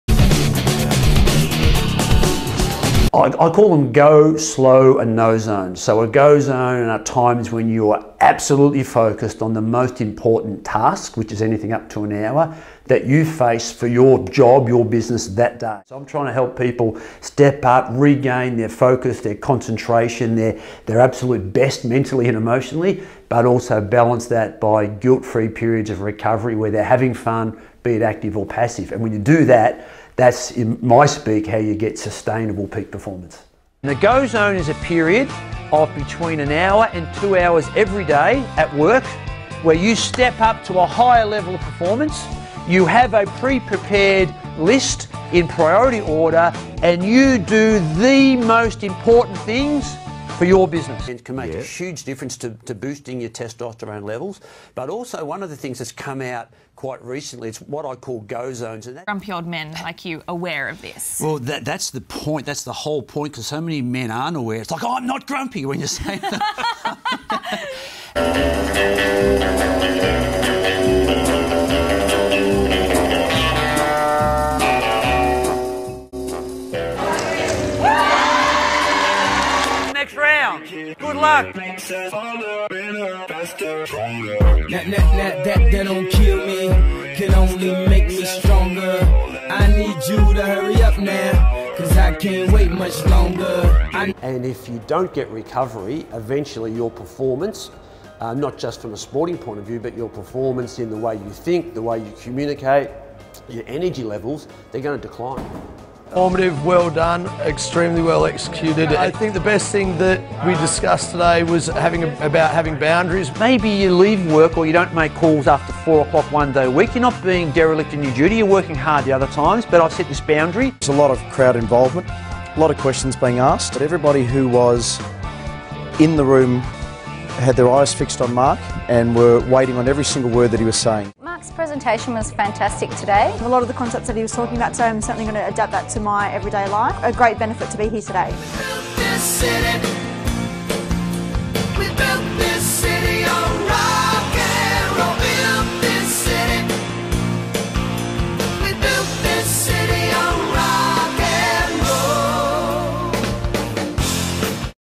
AAA 3 Minute Showreel Mark Mckeon.
Keynote, Conference and Media snippets with Mark Mckeon